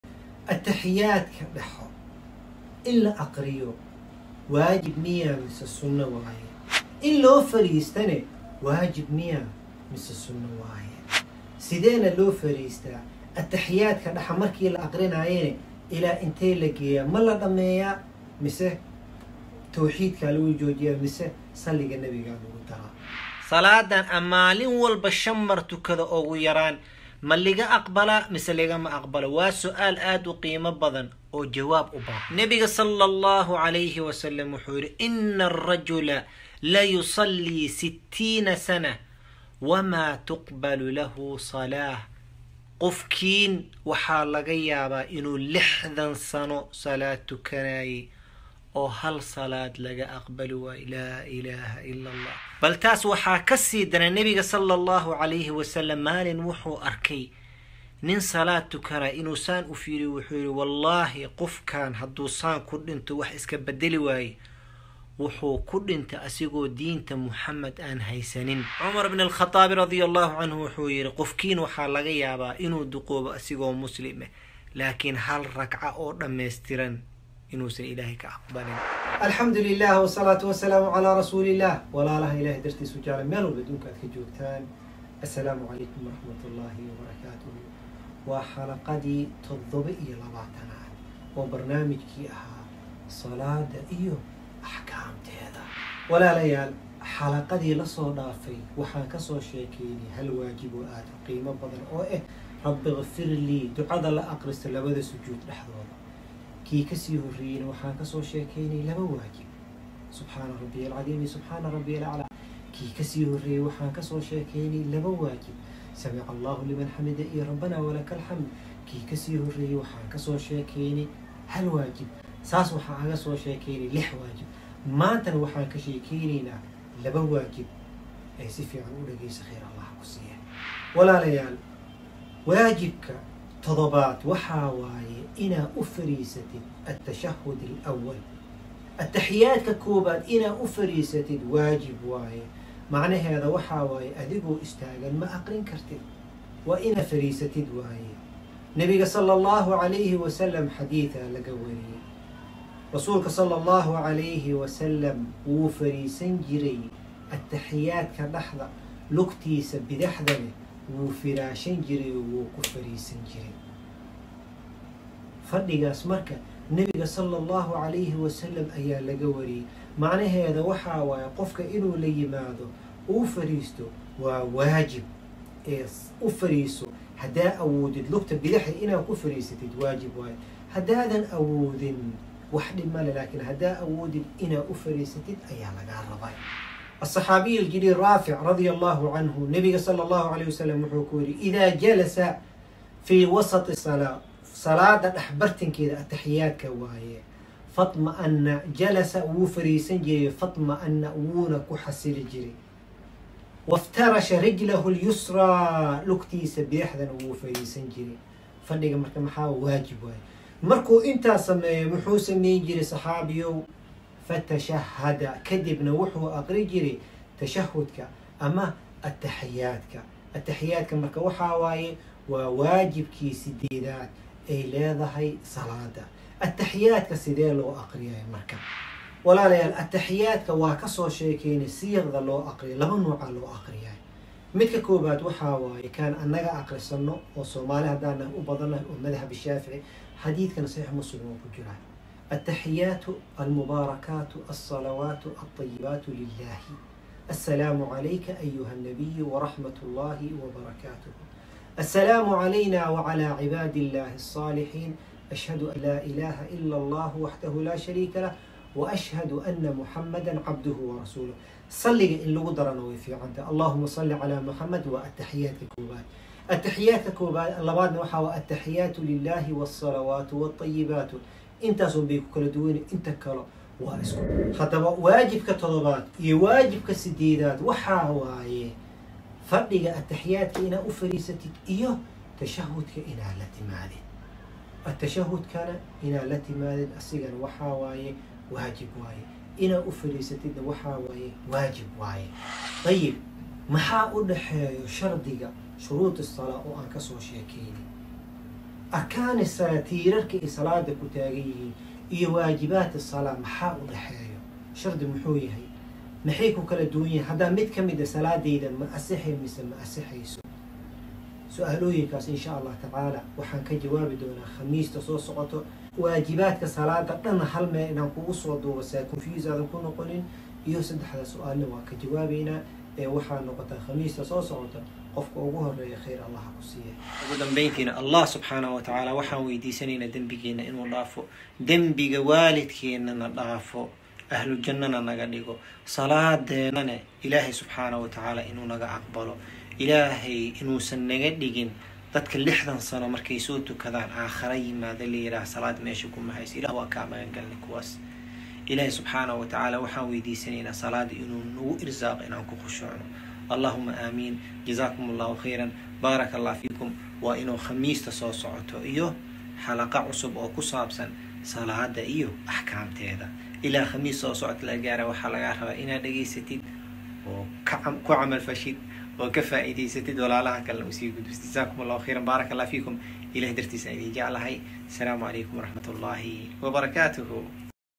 Qofkii Kas Uga Tago (التحيات) SALAADIISA Maxaa Ku Dhacayo?..(Q# 27)..:::Dr Ahmed Al-Yamaani
WAAJIBAADKA SALAADA WAXAA KA MID AH (TAKBIIRTA) :*SALAADA & AXKAAMTEEDA*(Q# 27)DR AHMED AL-YAMAANI